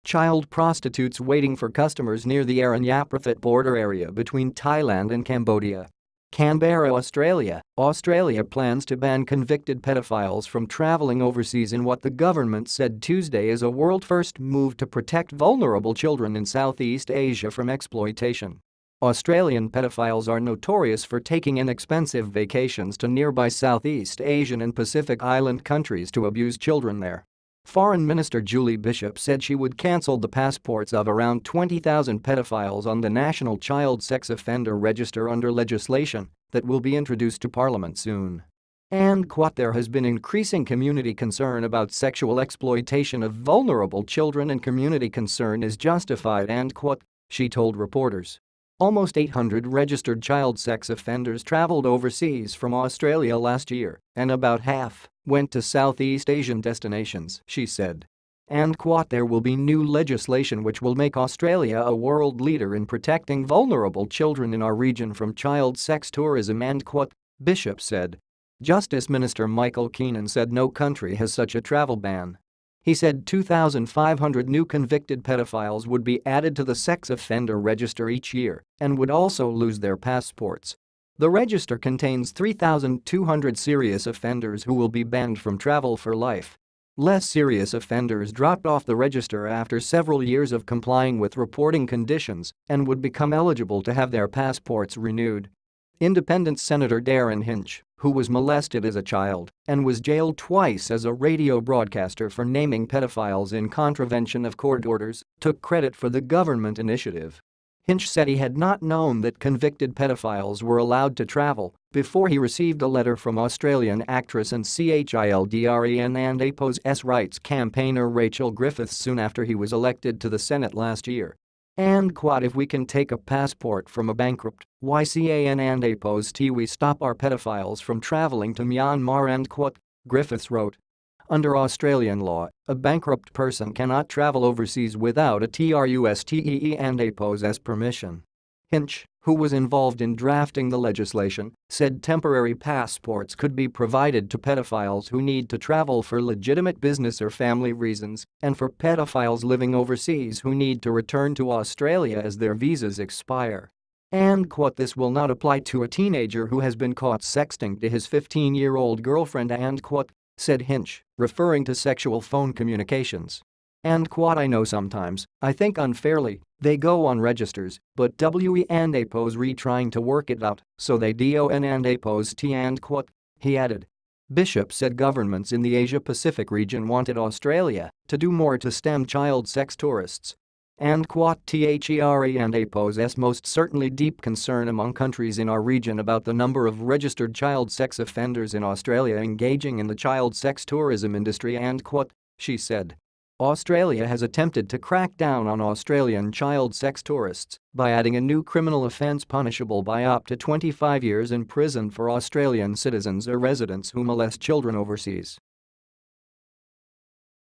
Australia Plans to Ban Pedophiles from Traveling Abroad in a Bid to Protect Asian Children
BREAKING NEWS: Australia Plans to Ban Pedophiles from Traveling Abroad in a Bid to Protect Asian Children